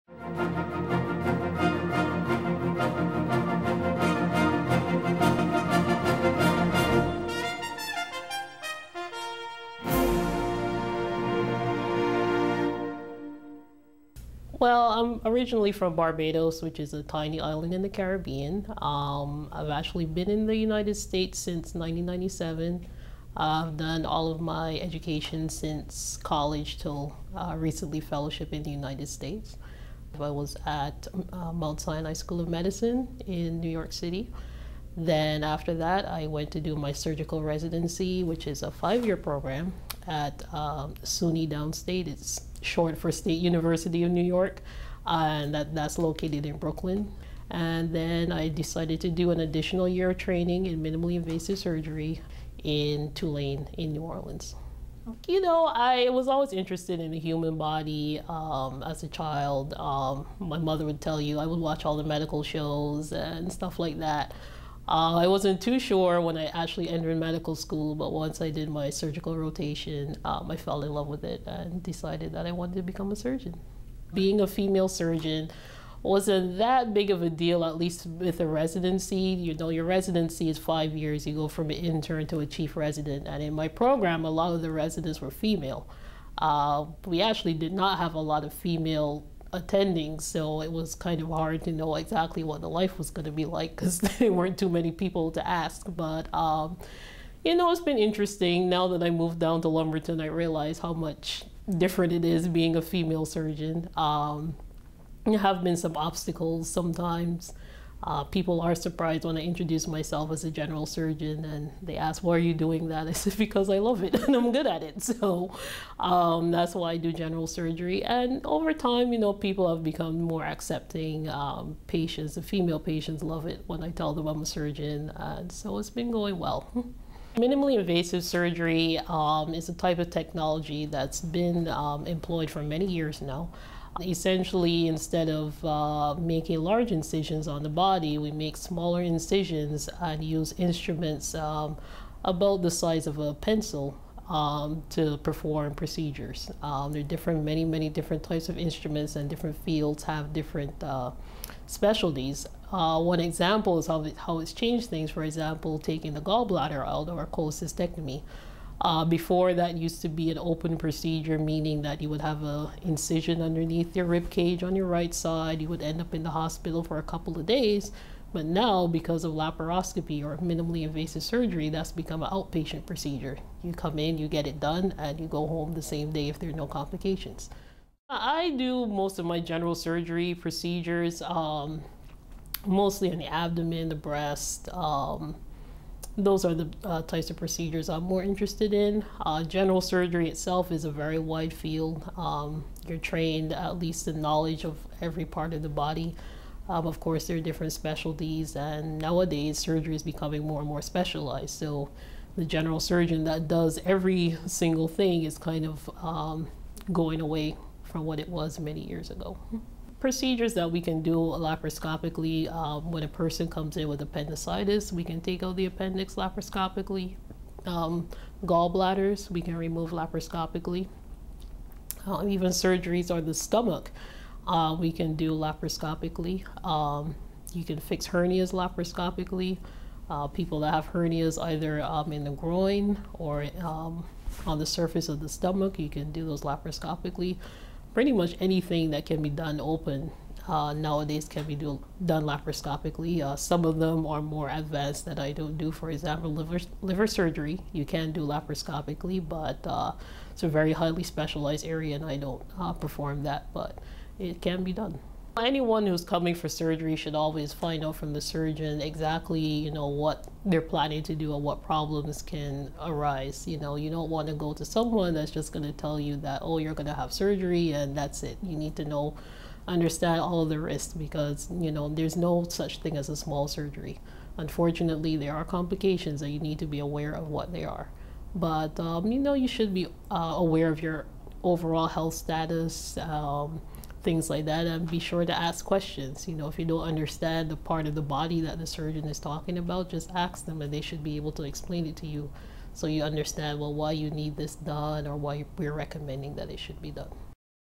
Meet Dr. Crichlow, General Surgeon
Dr. Lya Crichlow, of Southeastern Surgical Center, describes her educational background and training and her expertise, minimally-invasive surgery.